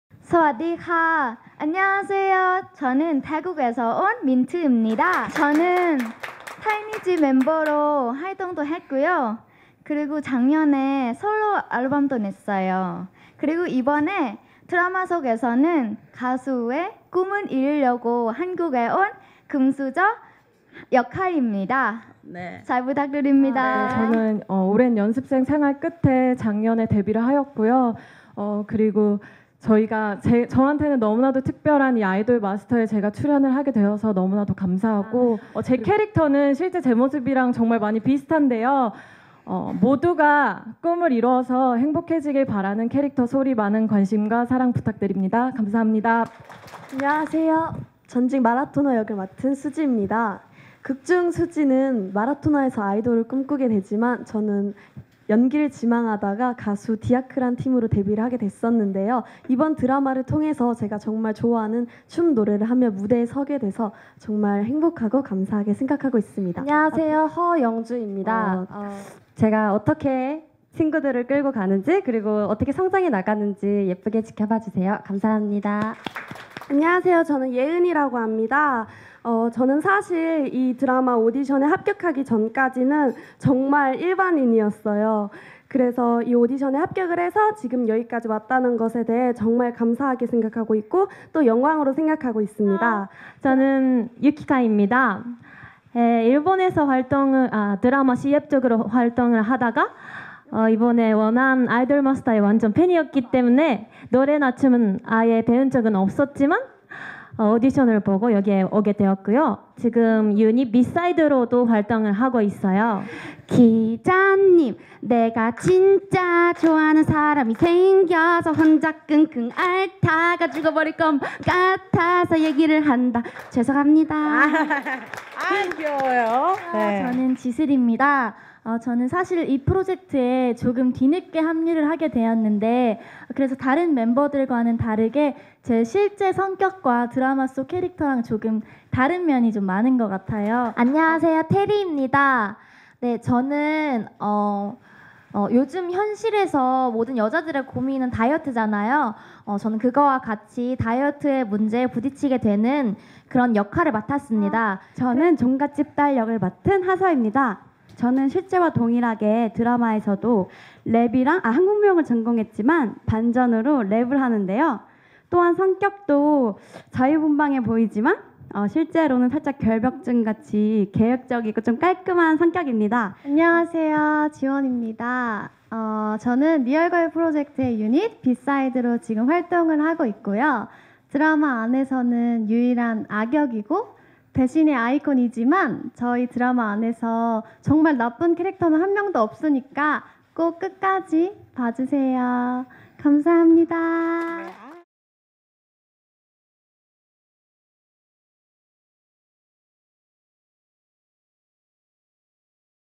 '아이돌마스터.KR(IDOL MASTER)' 11인 11색 상큼매력 소녀들 '지켜봐주세요' [MD동영상]
17일 오후 서울 소공동 롯데호텔에서 열린 케이블TV SBS funE 드라마 '아이돌마스터.KR'(극본 신혜미 연출 박찬율) 제작발표회에 참석한 11명의 소녀(민트, 소리, 수지, 영주, 예은, 유키카, 재인, 지슬, 태리, 하서, 지원)들이 개성넘치는 인사로 눈길을 끌었다.

'아이돌마스터.KR'은 아이돌을 꿈꾸는 소녀들의 성장 스토리를 통해 각박한 현실 사회에 희망의 메시지를 전하는 청춘 힐링 드라마로 프로듀서 강신혁 역에 성훈, 미스터리한 엔터테인먼트 대표 심민철 역에 박철민, 리얼걸프로젝트 연습생으로는 민트, 소리, 수지, 영주, 예은, 유키카, 재인, 지슬, 태리, 하서가 출연한다.